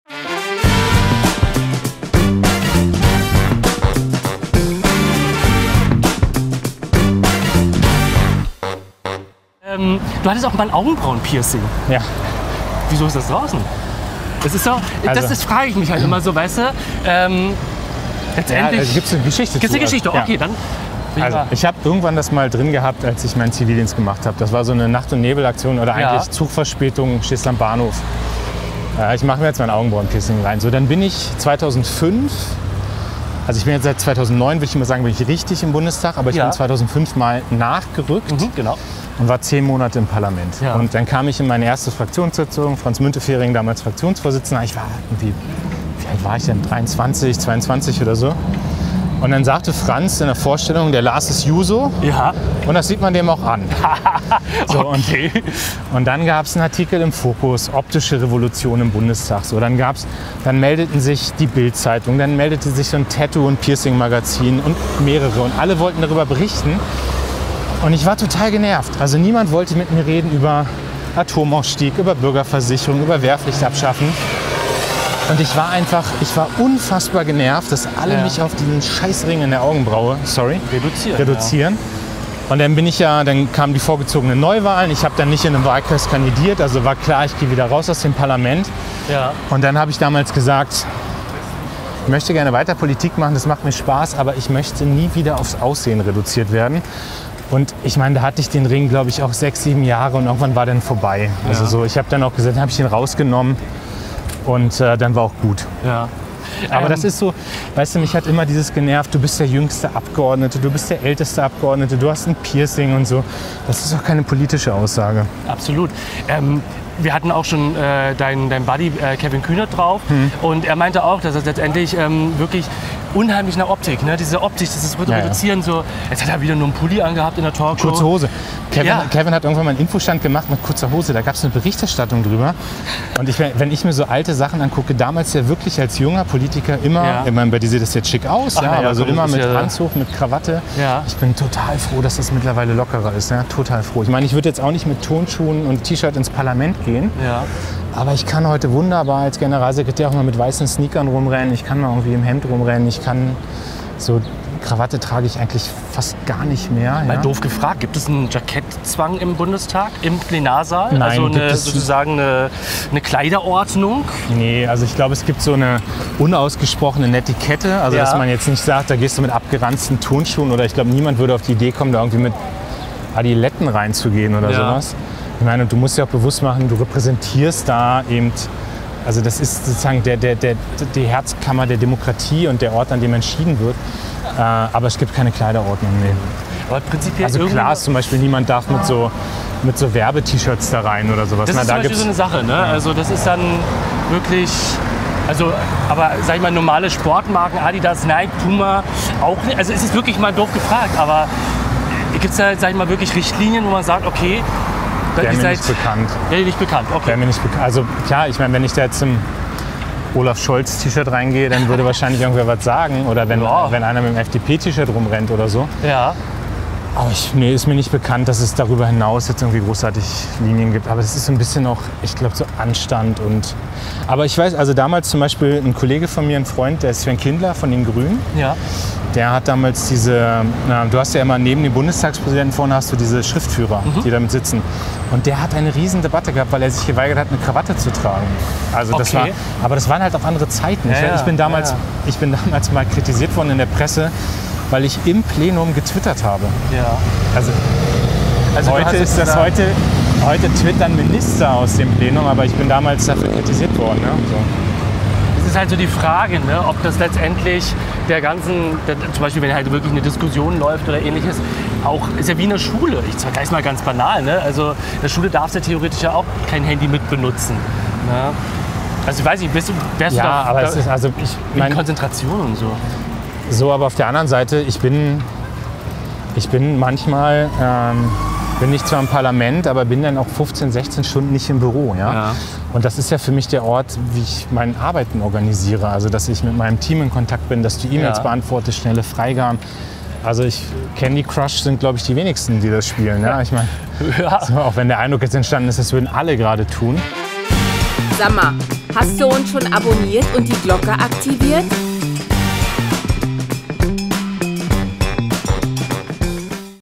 Ich hatte ein Piercing - Lars Klingbeil (SPD Generalsekretär) | VIP VELO
VIP VELO - Die klimaneutralste Talkshow der Welt - Gast : Lars Klingbeil - Generalsekretär der SPD